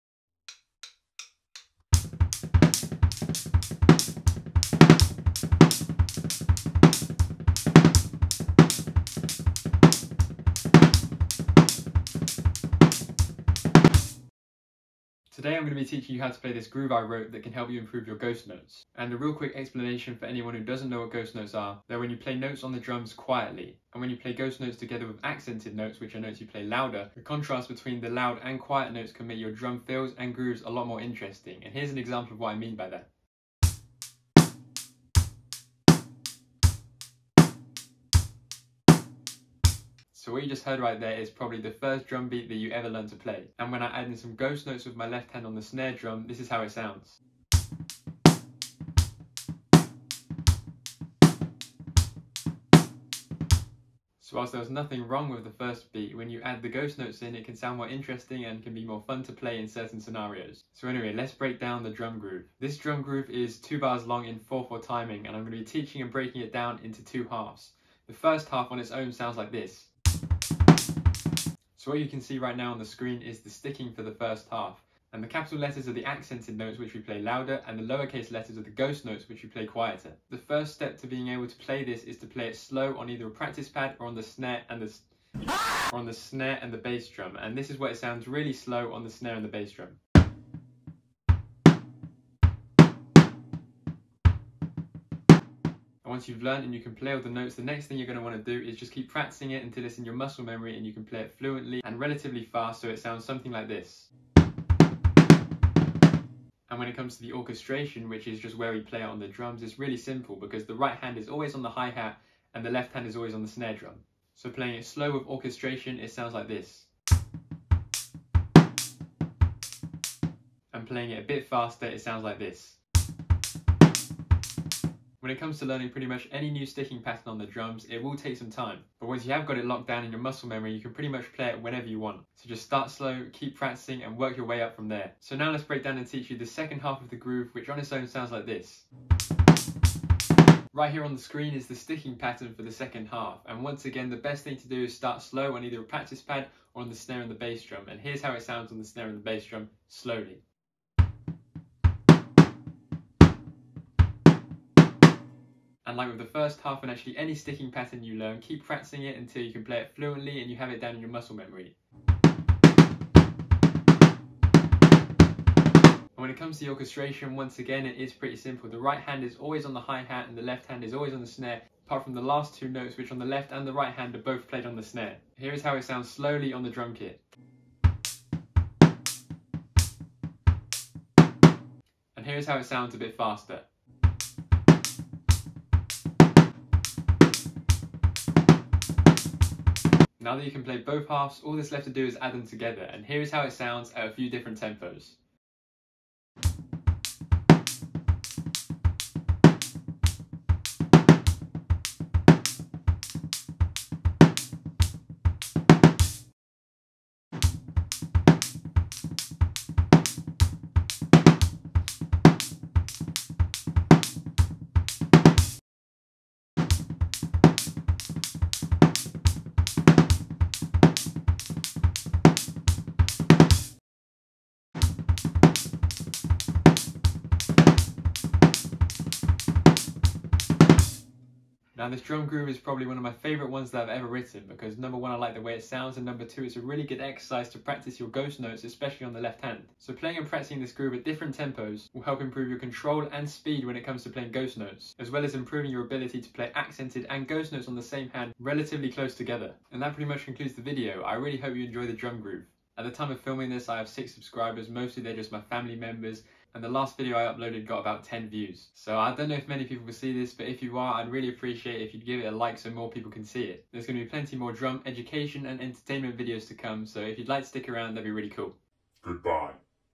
This EPIC Groove Will Improve Your Ghost Notes
Cool sounding drum groove that will help improve your ghost notes. Idk what else to say 😎